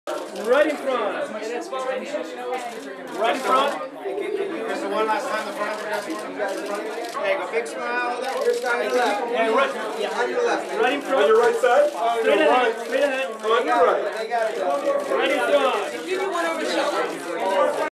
Christa B. Allen at Elle's 2nd Annual 'Women In Televisio...
Christa B. Allen at Elle's 2nd Annual 'Women In Television' Celebration 1/24/2013 in West Hollywood, CA.